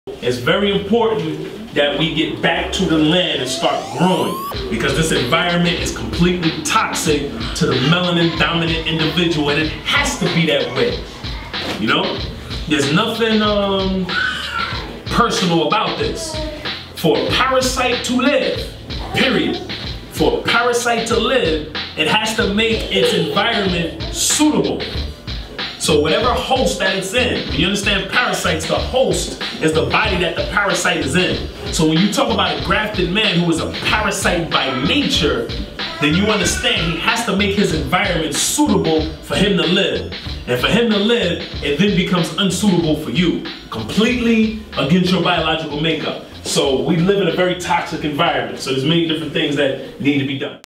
Born Master( Black Biology Tuesdays): "The melanin dominant individual"
During a Civilization class at Allah School in Mecca, Born Master builds on the toxic environment that melanin dominant people have to survive in.